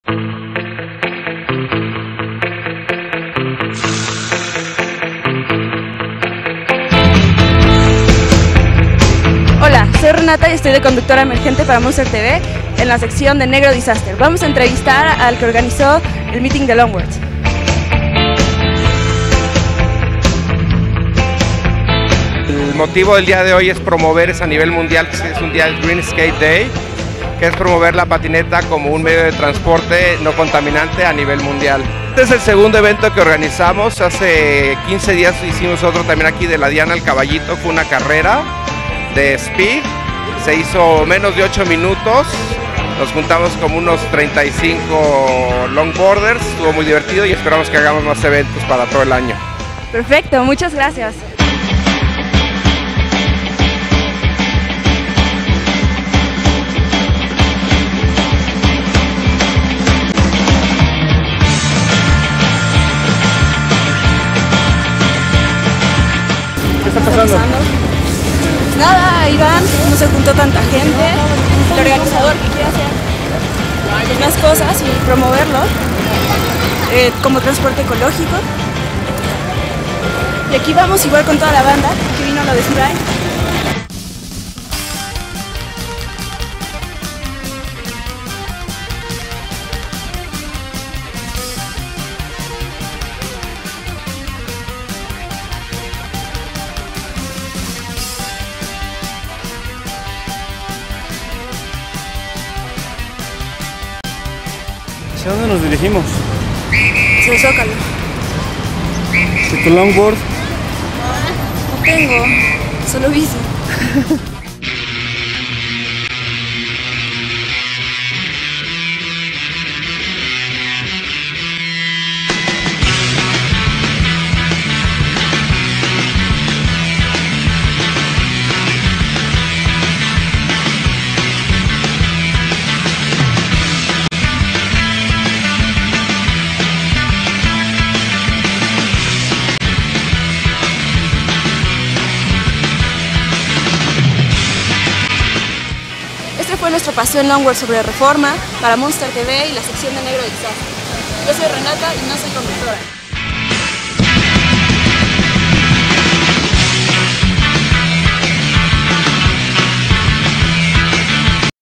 Longbord en Reforma (Sección Negro Disaster de Monster TV)
Nuestra sección Negro Disaster (Deportes) presenta un domingo de Longboard en Avenida Reforma, México D.F. Hostess Renatta Zissou